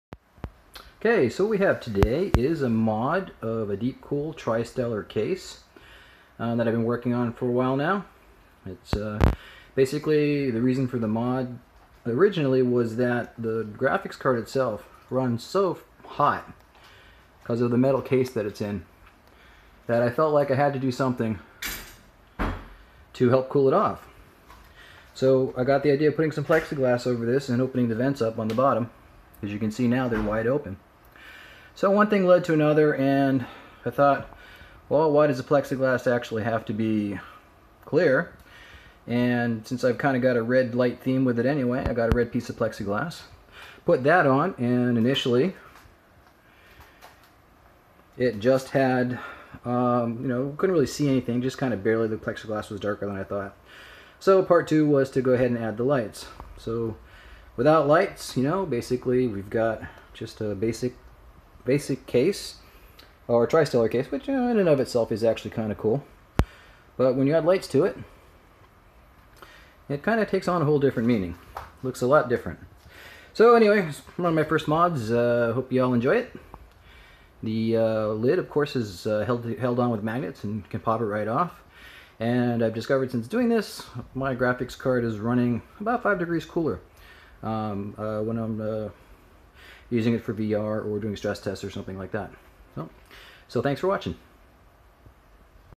Deep Cool TriStellar Graphics Pod Mod
A quick video of a mod I created for my TriStellar case. Similar to adding a plexiglass window to a standard case, I bought a piece online, had it formed, and painted the border so it looks like what you see.

I used two RGB light strips from the Deep Cool RGB 350 light set.

The GTX 1080 graphics card runs much cooler  now.

Parts used to create mod:
  * 15x16 inch dark red plexiglass
  * Gloss black enamel spray paint
  * Magnet tape .33mm thick from Home Depot
  * Deep Cool RGB 350 light set

System specs:
  * Gigabyte Aourus B450i Mini-ITX motherboard
  ^ AMD Ryzen 2600 CPU w/MasterLiquid Liter AIO liquid cooler
  * 12 GB Corsiar (1 8GB, 1 4GB 3000) memory
  * Corsair 240 GB Force LE SSD
  * Western Digital Blue 7200 1TB drive
  * PNY XLR8 GTX 1080 OC
  * Rosewill Hive 550s power supply
  * Arctic F12 case fan
  * Arctic f9 TC fan for graphics pod